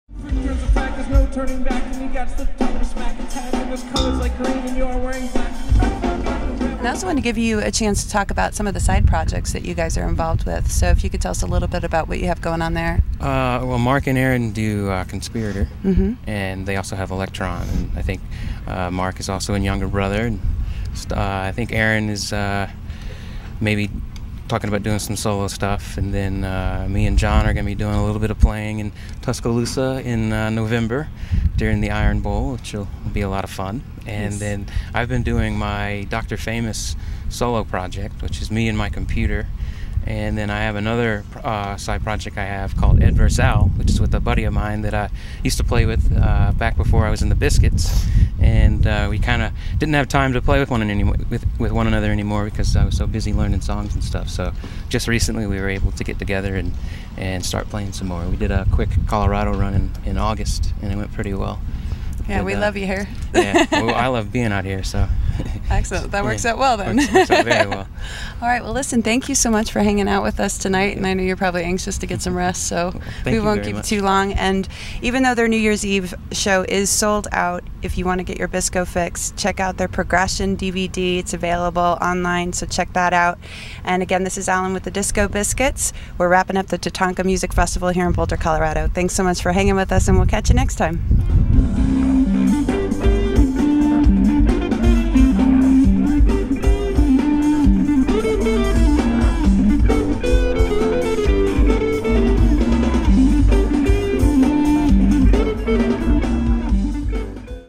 Disco Biscuits Allan Aucoin interview 2 "Side Projects"
Allen Aucoin, the drummer for the Disco Biscuits, talks about the bands side projects which include Conspirator, Electron, Younger Brother and some of his side projects including Dr. Famous.